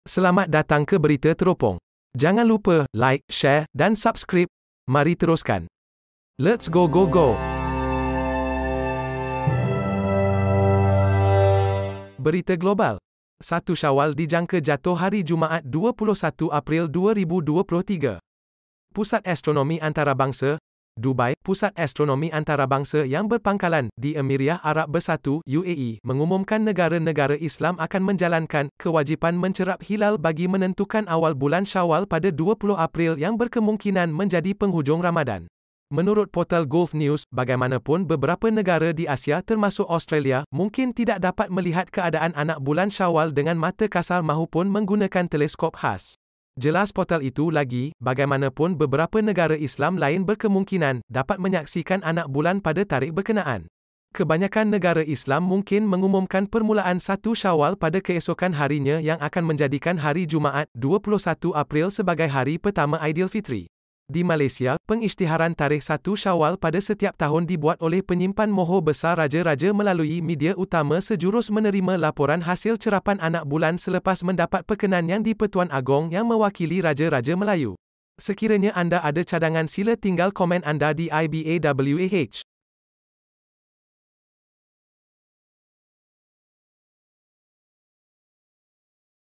21 April sebagai hari pertama Aidilfitri 2023
Kebanyakan negara Islam mungkin mengumumkan permulaan 1 Syawal awal,

raya terkejut lagi tahun ni